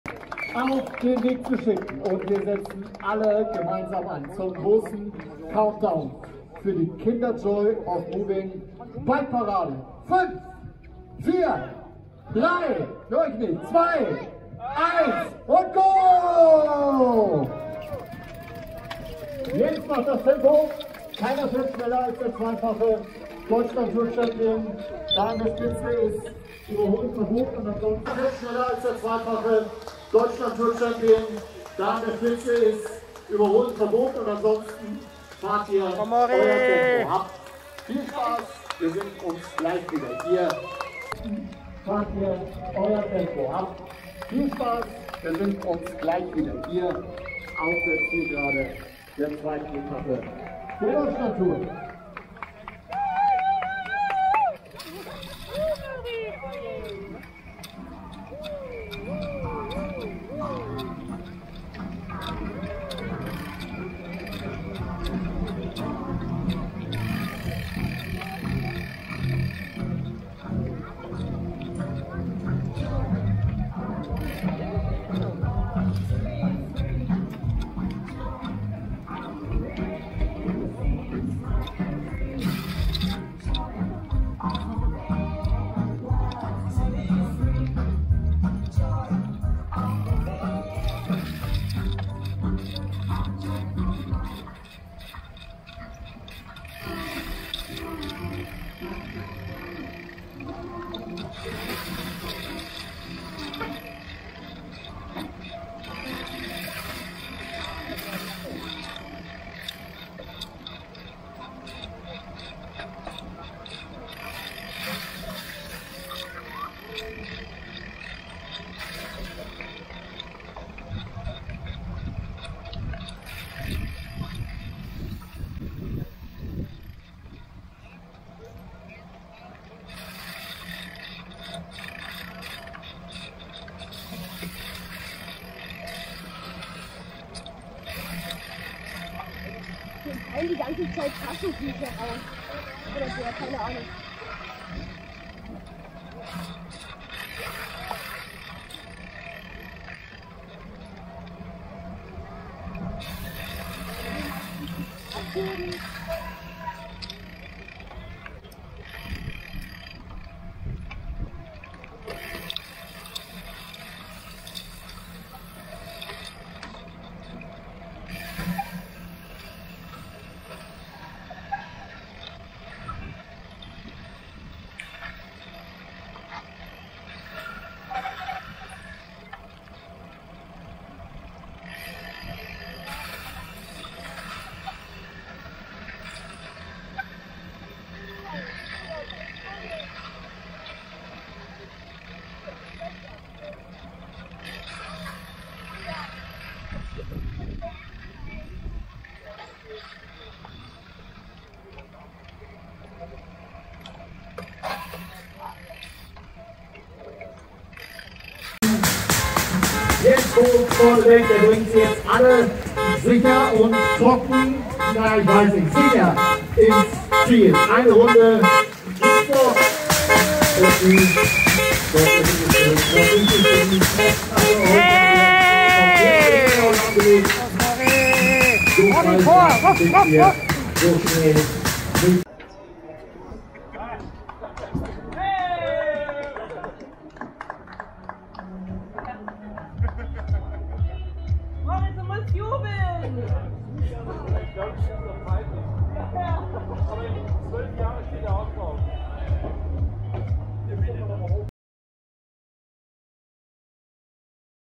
Deutschland Tour 2021 | Etappe 2 Ilmenau | Joy Ride mit Jens Voigt
Fahrer: Moritz Mikonya